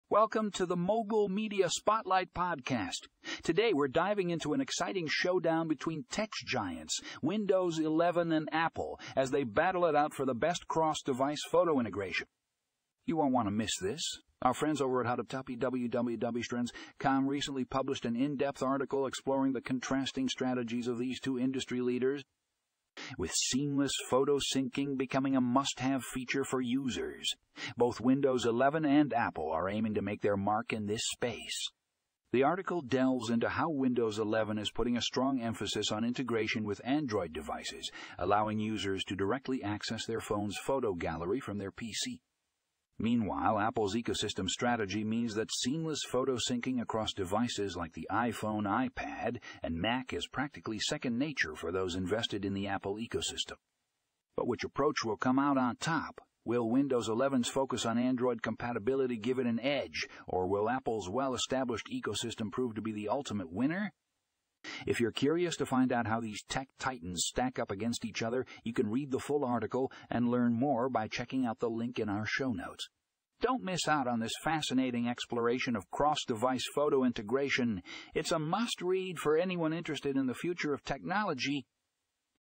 Windows 11 vs Apple Cross-Device Photo Integration: Tech Giants' Strategies Analyzed
Discover the contrasting strategies of tech giants Windows 11 and Apple as they compete for the best cross-device photo integration, focusing on Android compatibility and a seamless Apple ecosystem. Will one emerge as the clear winner? [Read More]